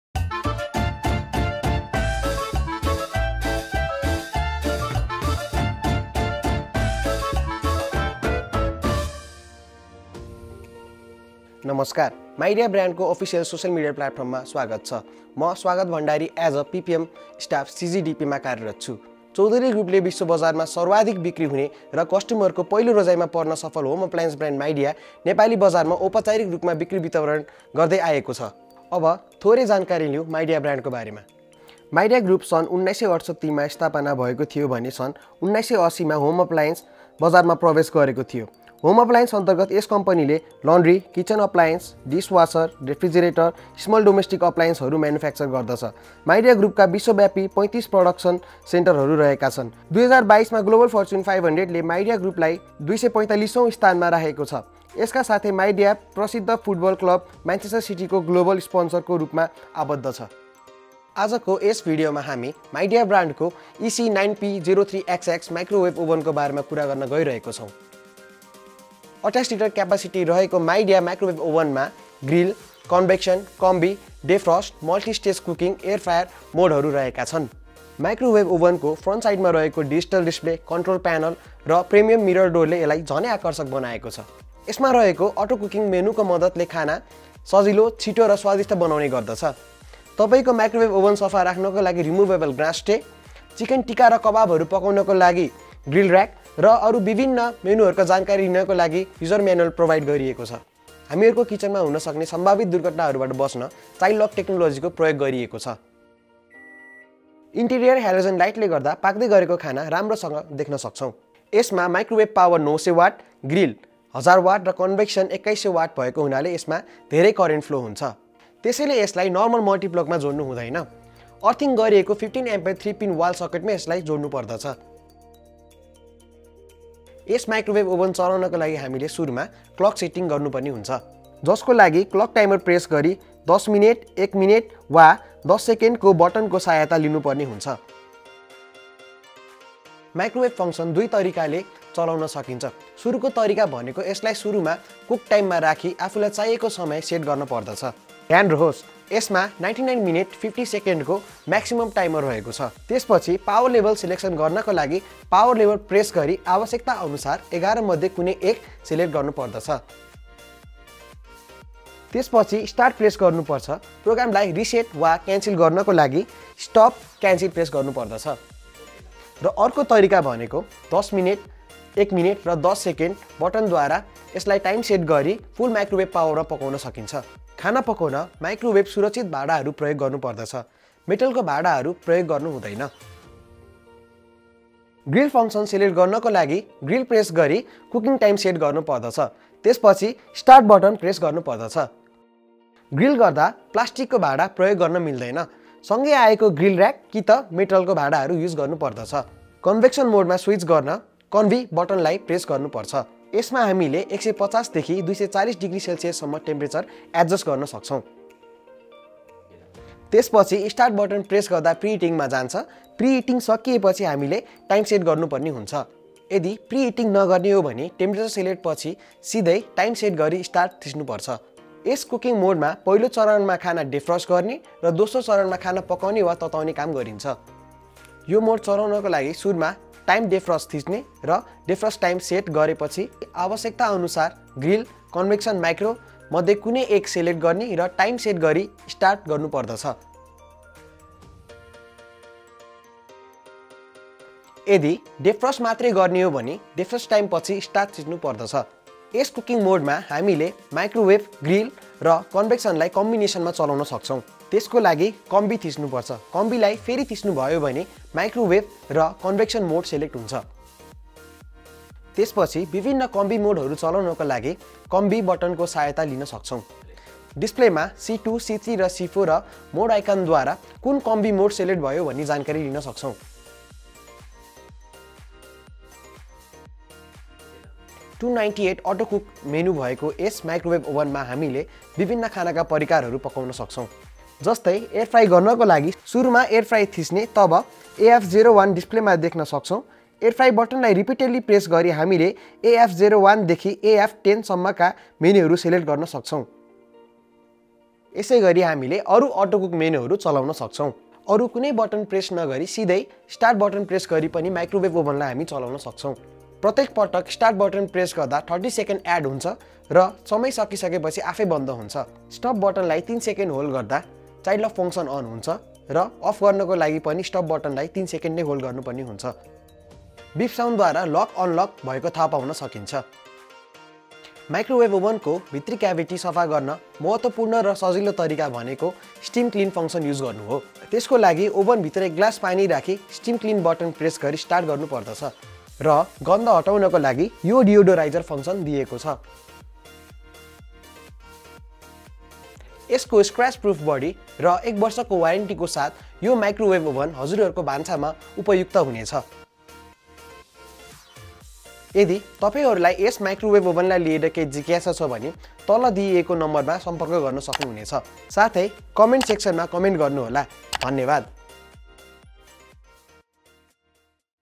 Midea 28L Microwave Oven | Effortlessly Cook and healthier solution for cooking | EC9P03XX-S00E00
If you're looking for a convenient and powerful way to cook and reheat your meals, look no further than the Midea 28L microwave oven. This countertop microwave features a powerful microwave, a defrost function, and an auto cook menu for effortless cooking and reheating. Plus, its child safety lock and easy-to-clean design make it a great choice for any household. With its quick meals and affordable price, the Midea 28L microwave oven is a must-have kitchen essential for any busy home. Join us and discover how to enjoy delicious and convenient meals with the Midea 28L microwave oven. EC9P02XX-S00E00 23L, MM720CGN 20L, AC0P042UP 31L, AC134AAI 34L